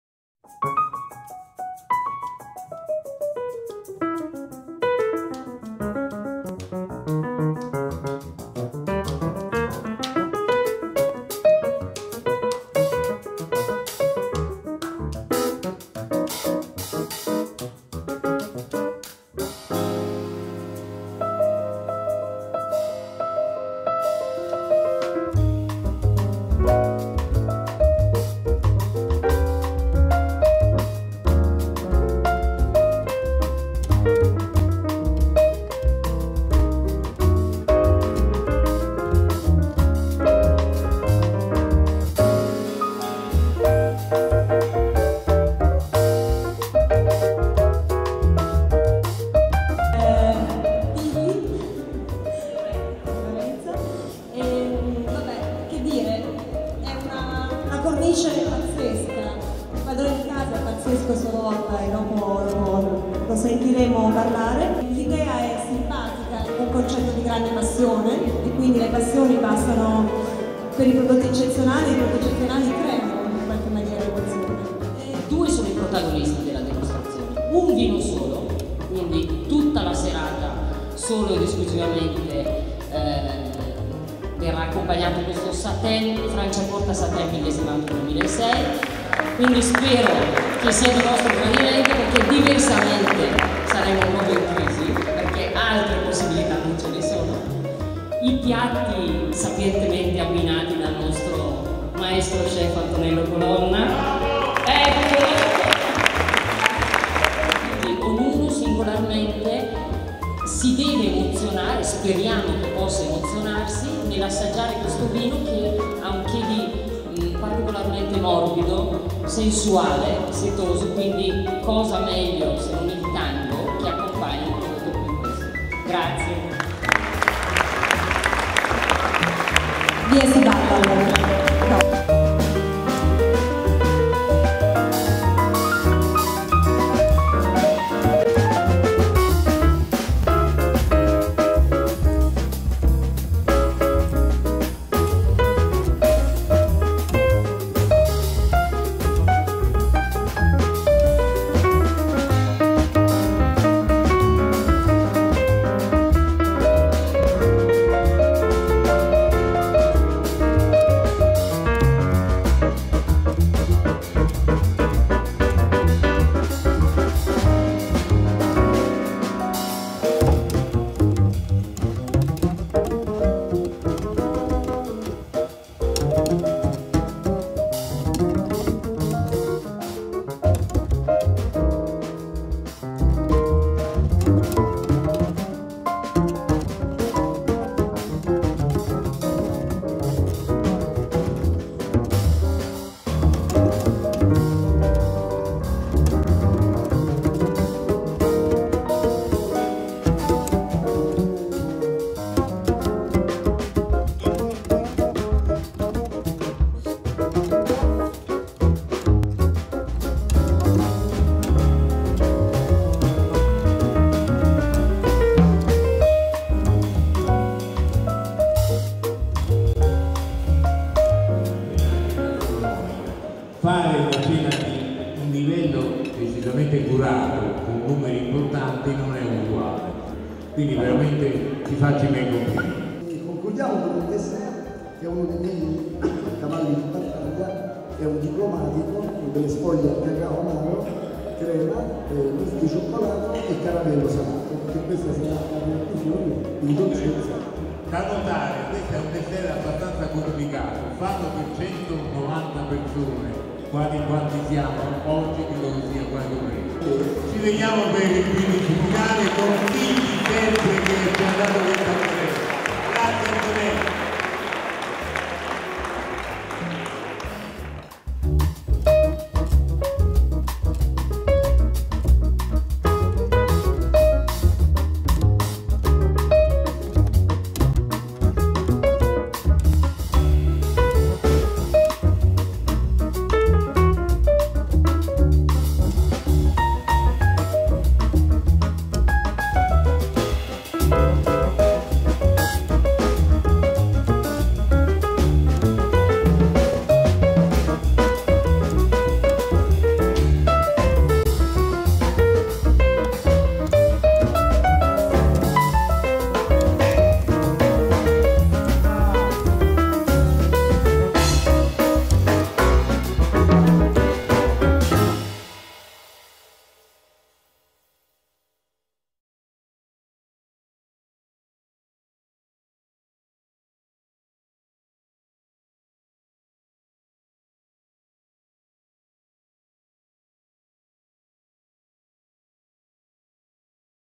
Tango bollicine e passioni gourmet
Reportage della serata "tango bollicine e passioni gourmet" organizzata dai fratelli Berlucchi in occasione della presentazione di un millesimato 2006.
Lo chef Antonio Colonna accompagna con un menù di grande levatura, il tutto viene incorniciato con l' esibizione di due ballerini di tango.
L'evento è stato organizzato da Witaly presso l'Open colonna in Via Nazionale a Roma.

shooting and editing by Andrea D'Amata